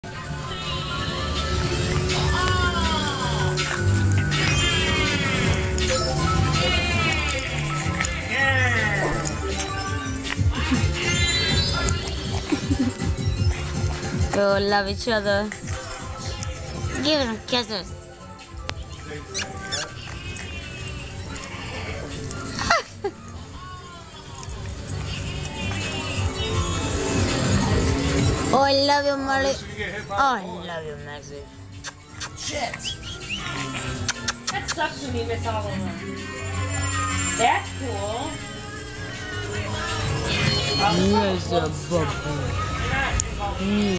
Dog Play Boston Terrier playing with Pitbull/Jack Russell Terrier Mix
Marley the Boston Terrier plays with Max the Jack Russell Terrier/Pitbull mix